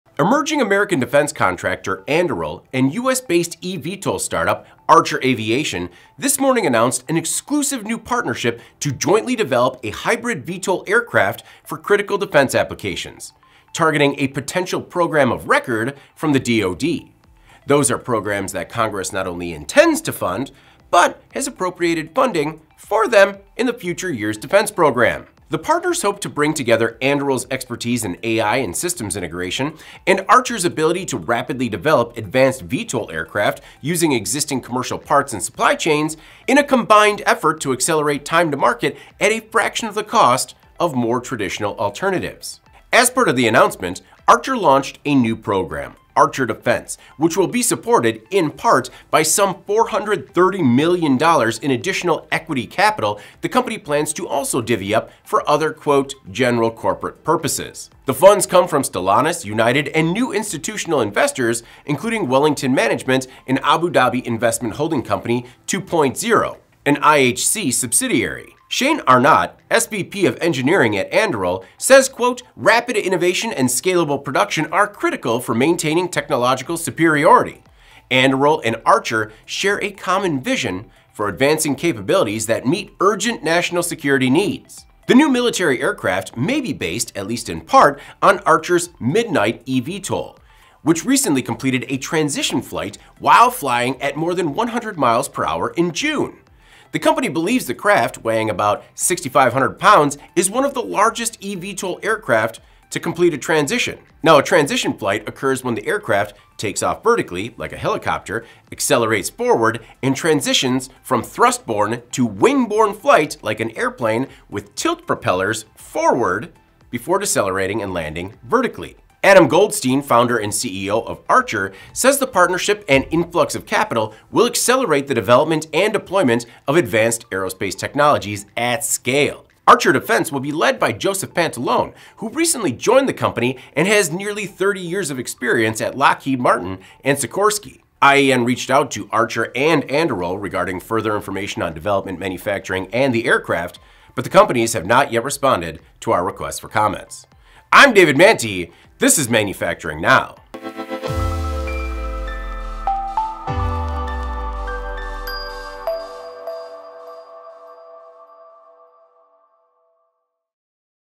Anduril, Archer to Make New Military Aircraft for Critical Defense Applications
Emerging American defense contractor Anduril and U.S.-based eVTOL startup Archer Aviation this morning announced an exclusive new partnership to jointly develop a hybrid VTOL aircraft for critical defense applications, targeting a potential program of record from the DOD. Those are programs that Congress not only intends to fund but has appropriated funding for them in the Future Years Defense Program.